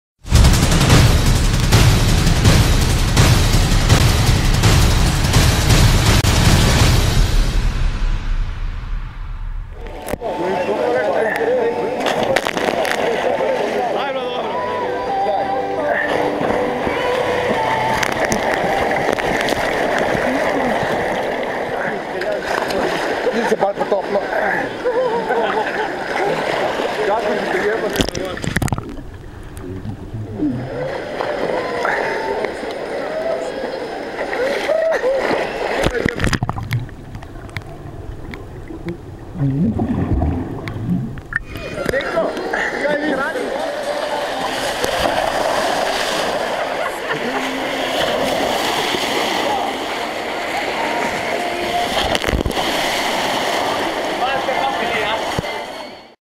Rokometaši v Thermani Laško
Naši fantje so si po napornih treningih privoščili sprostitev v Thermani Laško, kjer so uživali v vodnih radostih termalnega parka. Več v fotogaleriji na